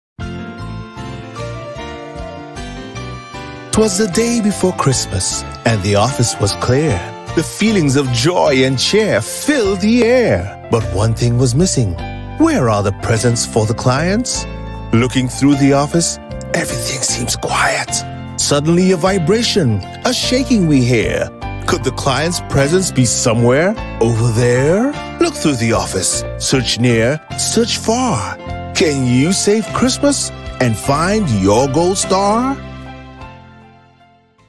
Where's My Gold Star?
Twas the day before Christmas
and the office was clear,
The feelings of joy and cheer filled the air.
But one thing was missing,
"Where are the presents for the clients?"
Looking through the office,
everything seems quiet.
Suddenly a vibration, a shaking we hear,
Could the client's presents be somewhere over there?
Look through the office,
Search near and search far,
Can you save Christmas and find your gold star?